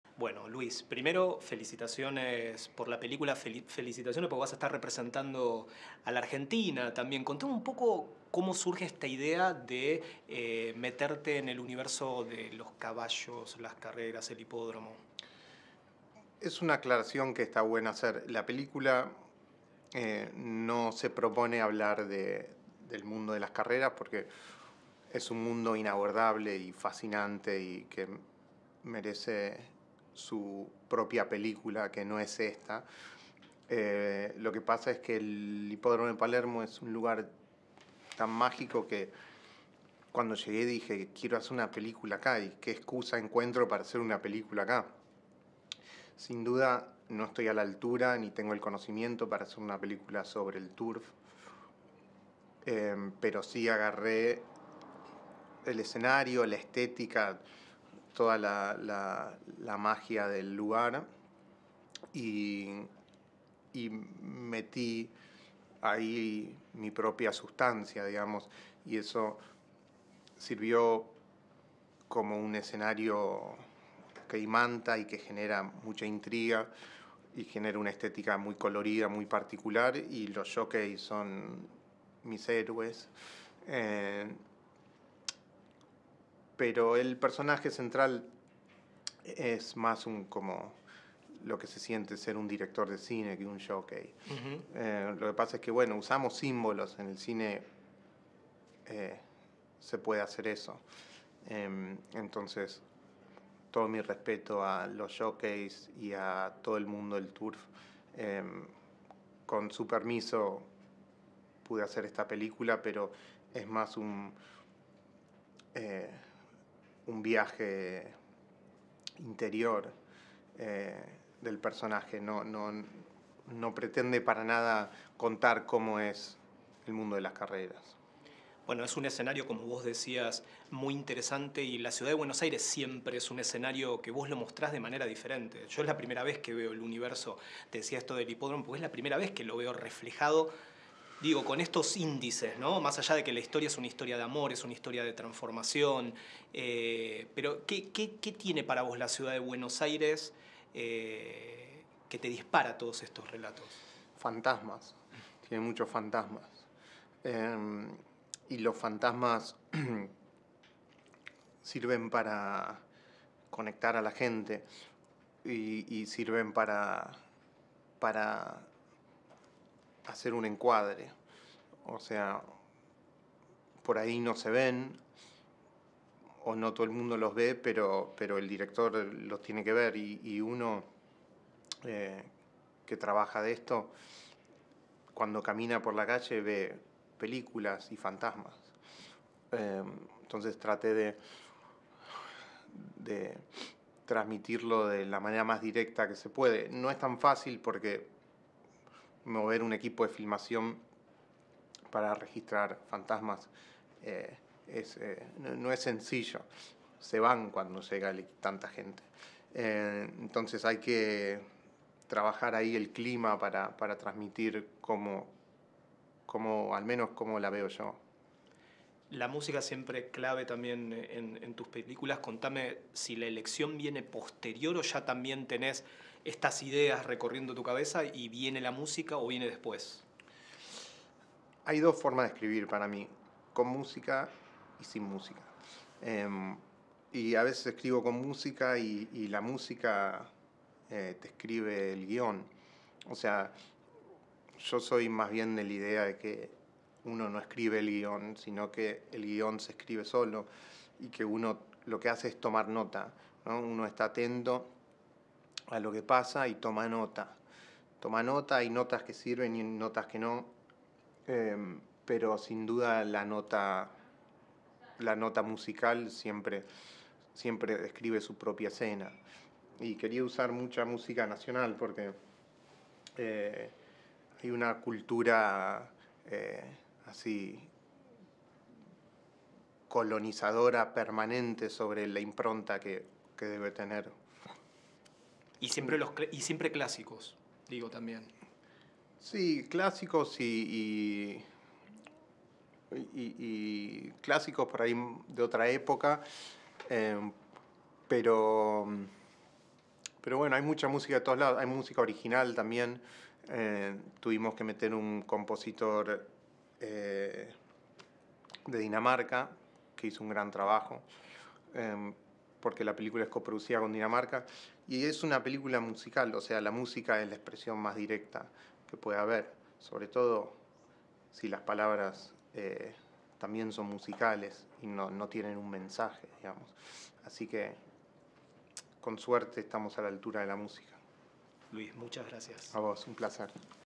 Entrevista a Luis Ortega, director de EL JOCKEY
El realizador habla de la película que representa a Argentina en los Premios Goya y Premios Oscar